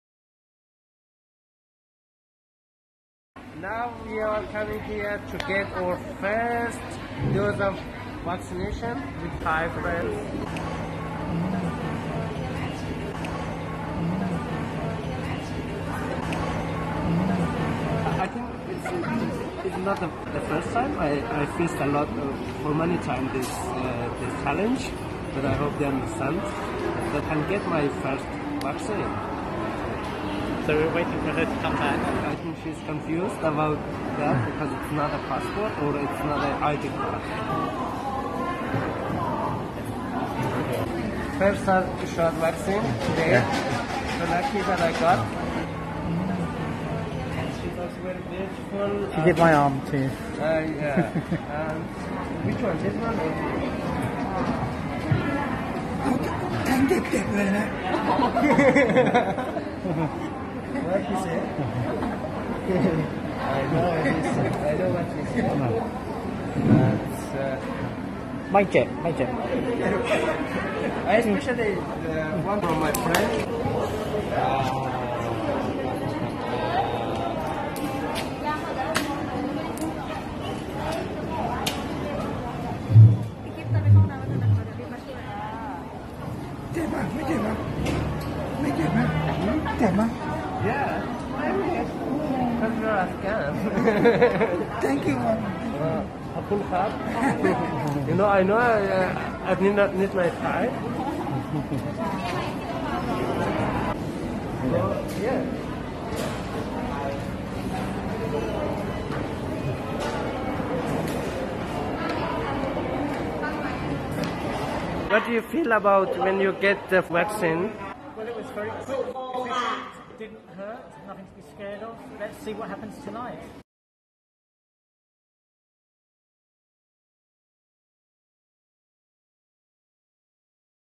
Getting the first Jab of Covid-19 vaccine. اولین دوز واکسین کرونا را دریافت کردم
one of Thai friend manage for me vaccine and I feel more safe when I get first jab of vaccine.